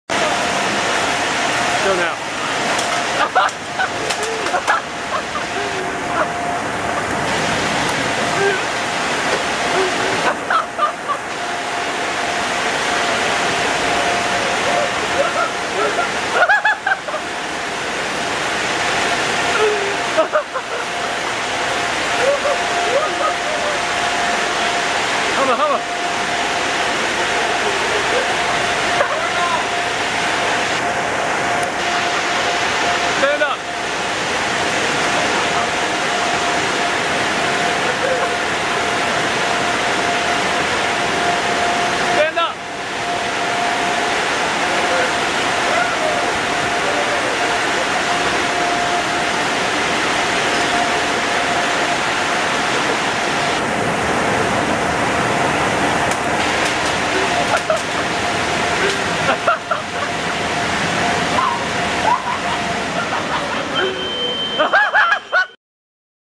Alex vs.The Auto Dryer
Alex ventures into a  car wash for the second time, this time he battles another blow dryer.